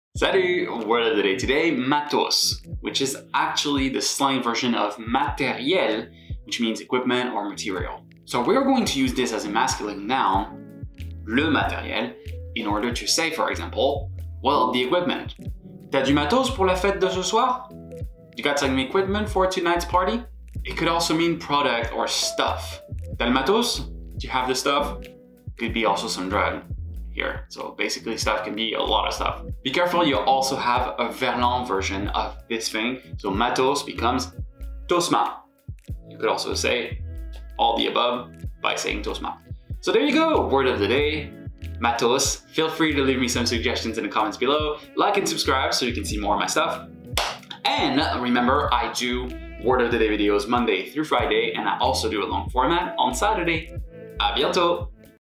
MATOS - French Slang Pronunciation & Examples
Matos, like many slang words, is a normal french word with a modified ending.

00:00:00 - matos
[matos]
Slang version of matériel – equipment

MASC. NOUN
00:00:08 - Equipment

T’as du matos pour la fête de ce soir ?
Do you have some equipment for tonight’s party?

00:00:20 - Product/stuff

T’as le matos ?
You have the stuff?

Verlan: tosma.
If you're interested in taking classes with me or checking out additional materials, here's my